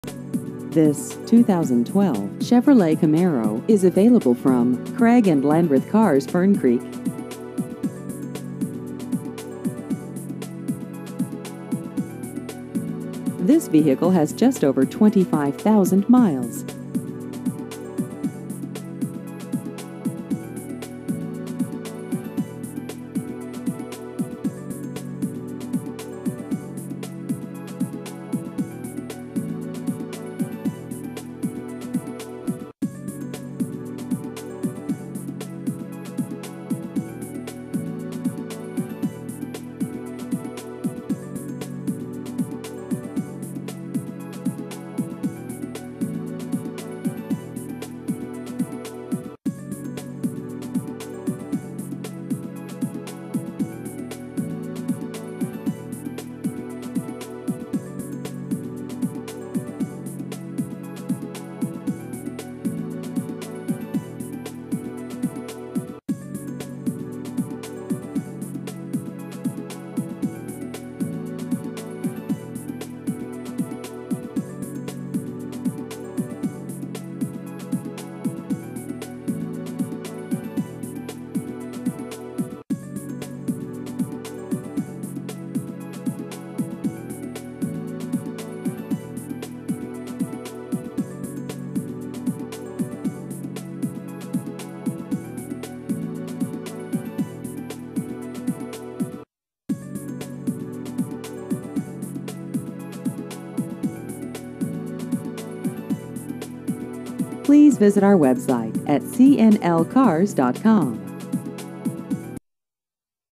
2012 Chevrolet Camaro Used Cars Louisville KY
This 2012 Chevrolet Camaro is available from Craig and Landreth Cars - Fern Creek.